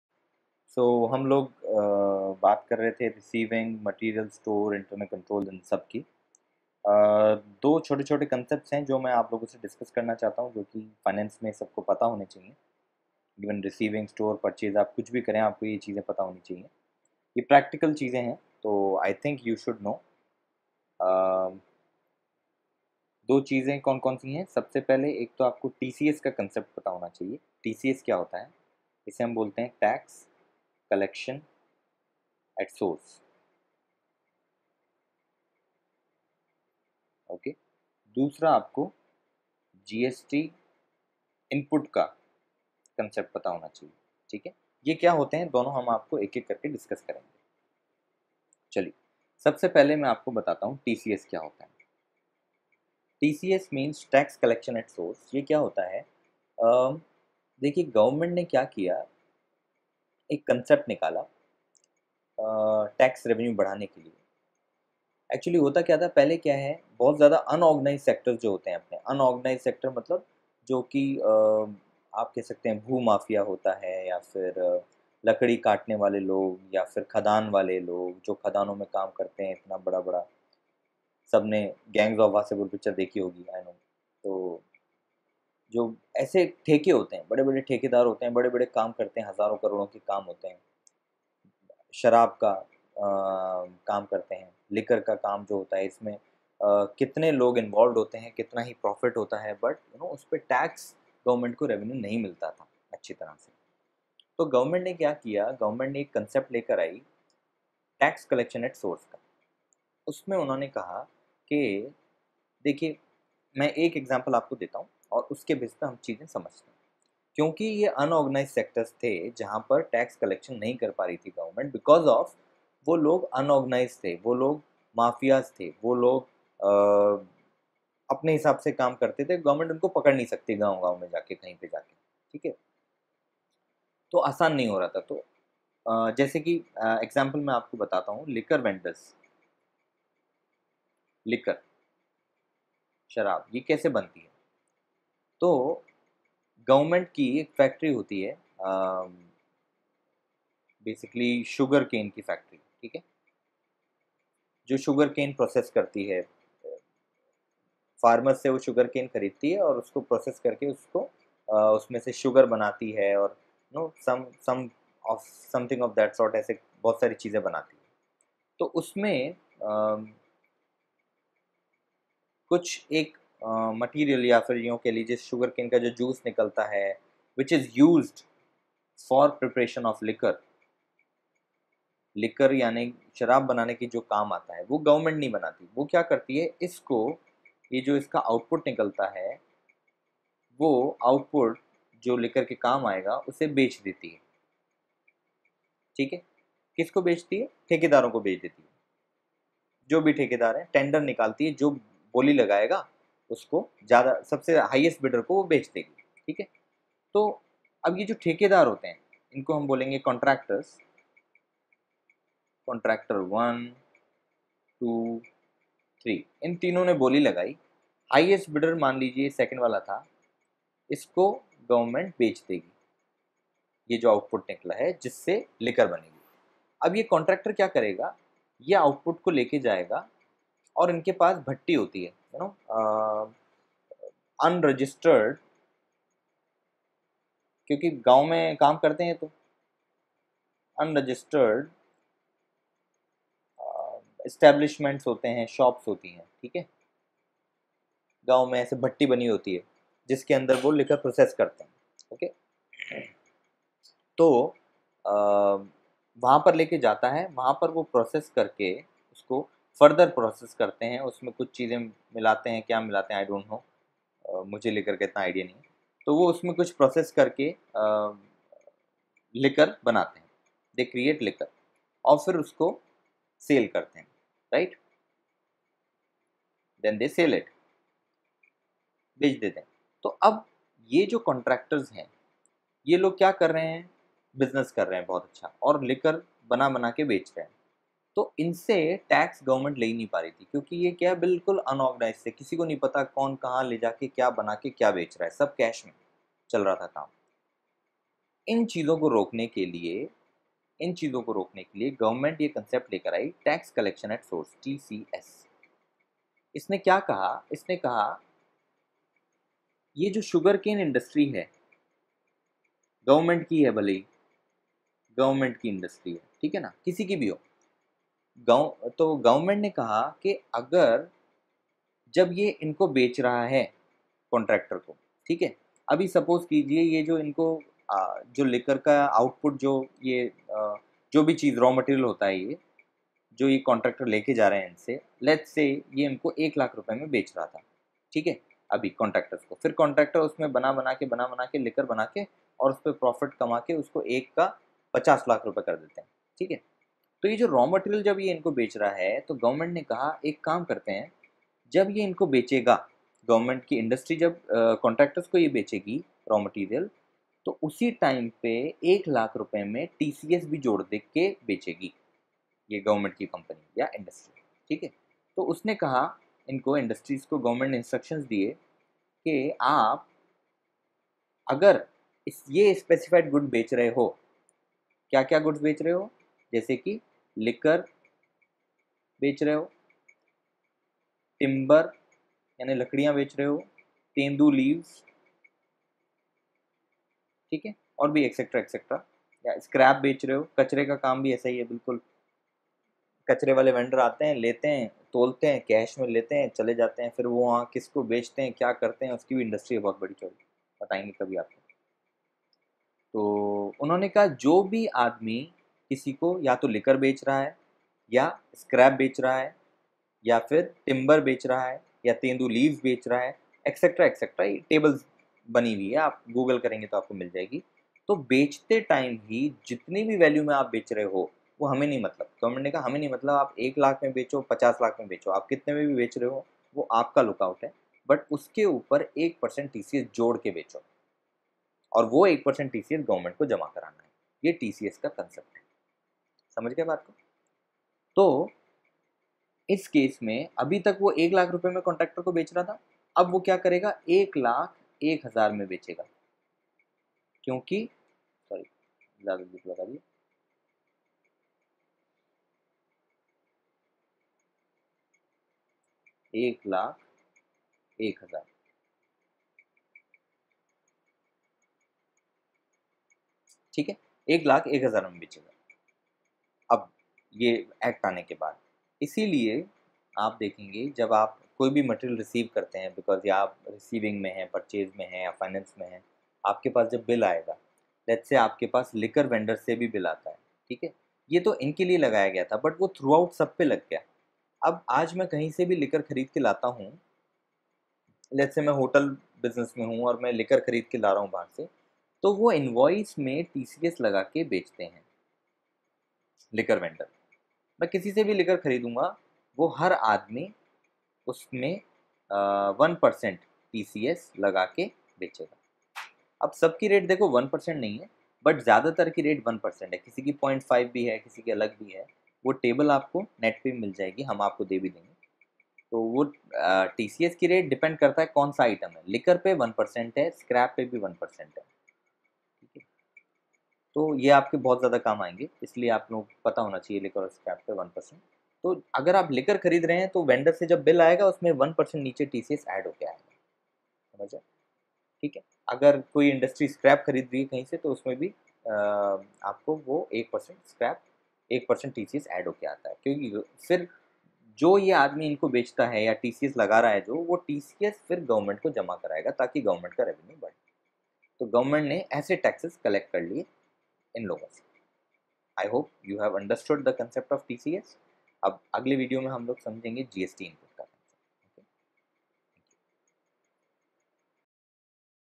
MATERIAL MANAGEMENT -ACCOUNTING FOR GOODS - TCS
How to account for goods received with implication of GST input, accounting entries, TCS (tax collection at source. How does the Internal control system works for goods received, how to verify an invoice for goods received.
For all these answers take full course